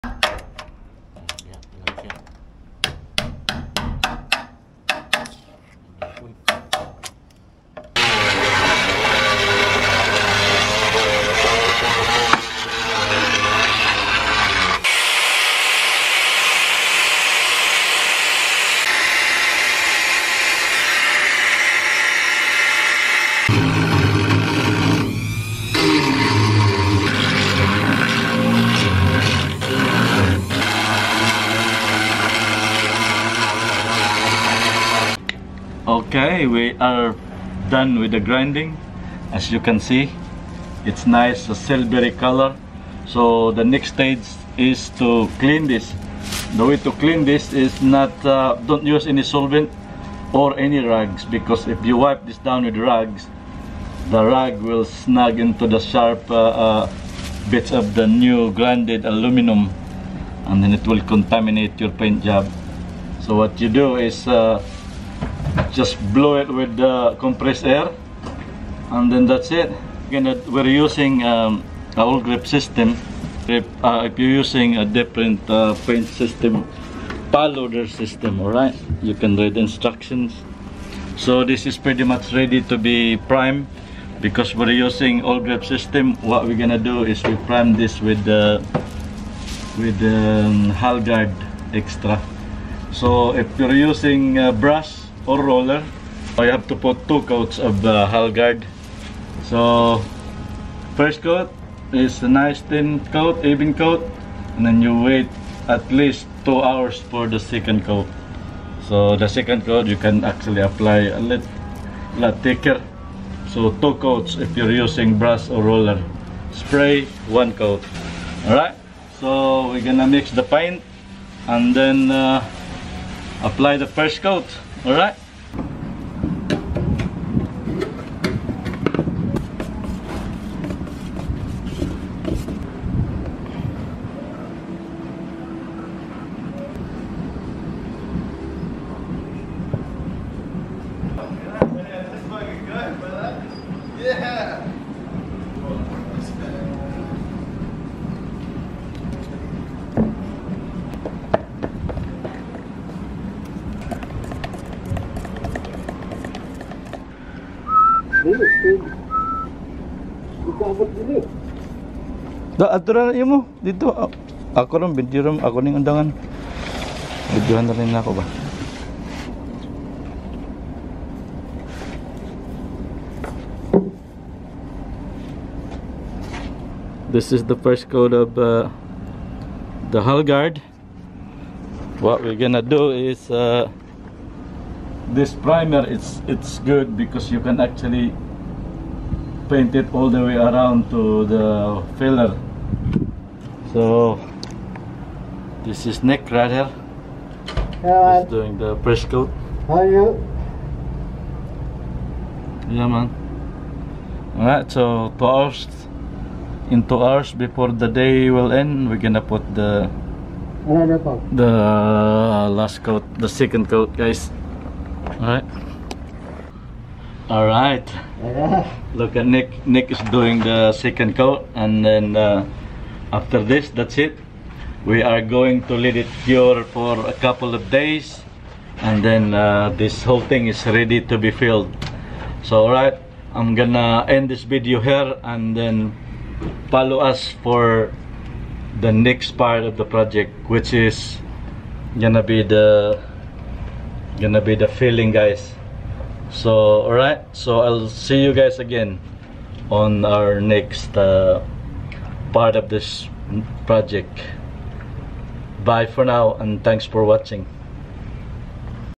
Aluminum Corrosion Cure: Hullgard Primer by Awlgrip
"Unlock the ultimate solution for aluminum corrosion control in our latest video tutorial. Watch as we guide you through the comprehensive process of treating corrosion on aluminum surfaces and prime them to perfection using Hullgard by Awlgrip. From identifying corrosion trouble spots to applying the industry-leading Hullgard primer, this step-by-step video will equip you with the knowledge and skills needed to do your painting projects onboard. Say goodbye to corrosion worries and hello to a durable, long-lasting finish."